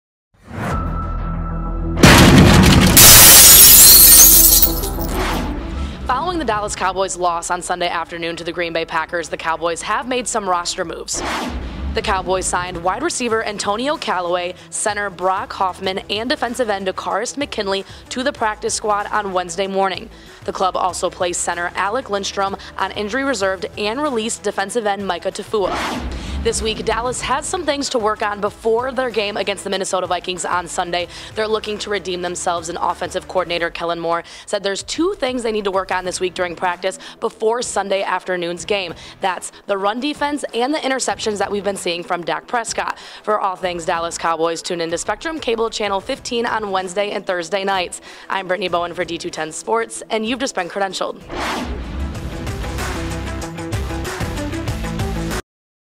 NFL Week 11: Dallas Cowboys Roster Moves
The Dallas Cowboys signed WR Antonio Callaway, C Brock Hoffman and DE Takkarist McKinley to the practice squad on Wednesday.

The club also placed C Alec Lindstrom on Practice Squad/Injured and released DE Mika Tafua from the practice squad.